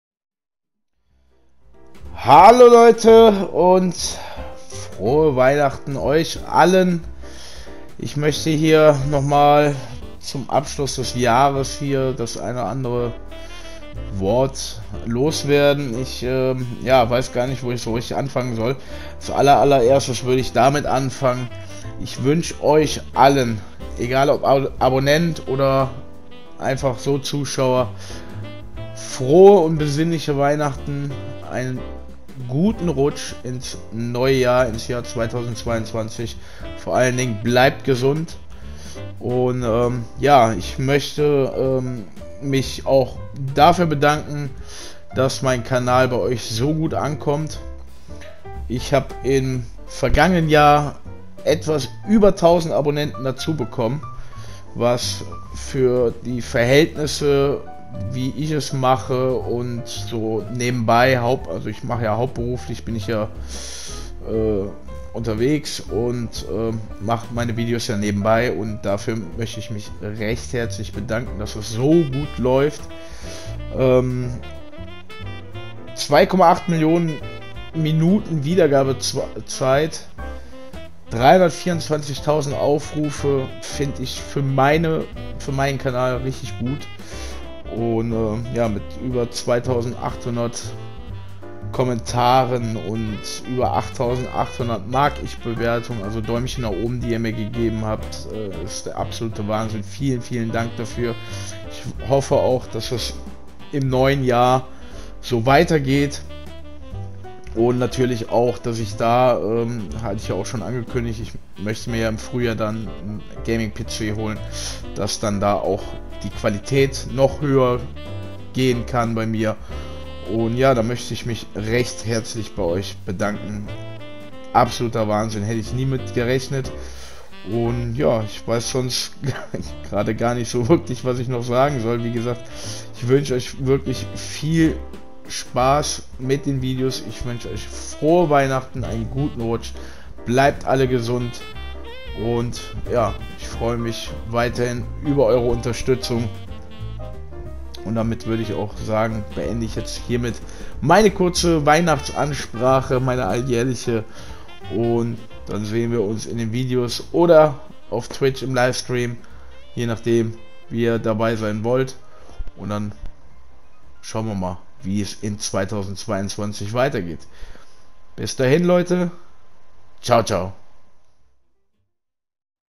Danke an alle Abonnenten und Zuschauer für ein tolles Jahr 🎅🎄😍
Hallo Leute,

ich wünsche Euch allen Frohe Weihnachten und einen guten Rutsch ins Jahr 2022, aber vor allem bleibt gesund.

Freue mich auf das Jahr 2022 mit Euch.

Euer Scheiben-Willi